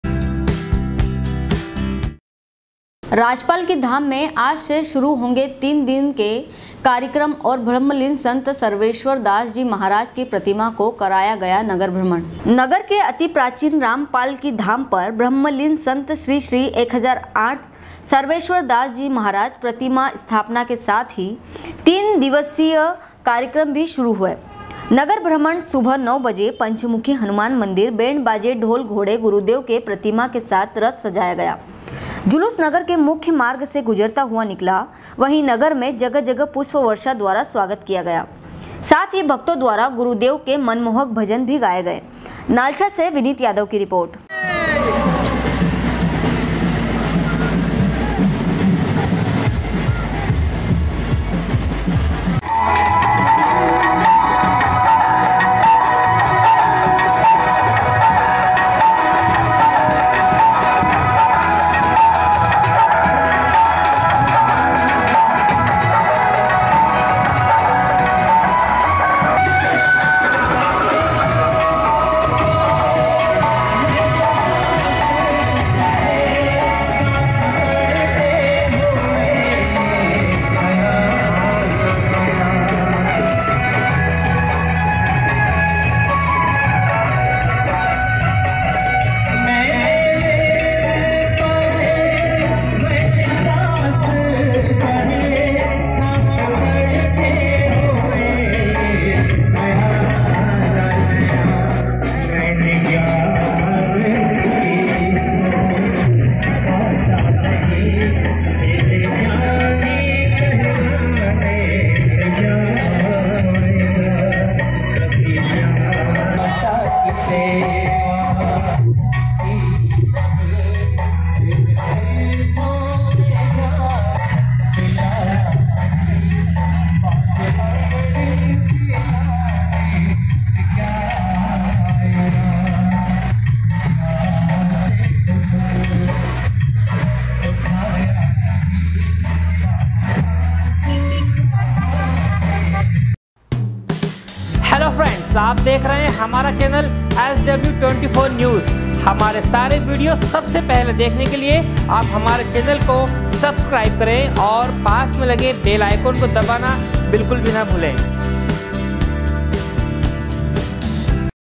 sw24news nalcha, 20.02.2020
रामपाल की धाम में आज से शुरू होंगे तीन दिन के कार्यक्रम और ब्रह्मलीन संत सर्वेश्वर दास जी महाराज की प्रतिमा को कराया गया नगर भ्रमण नालछा से विनीत यादव की रिपोर्ट#sw24news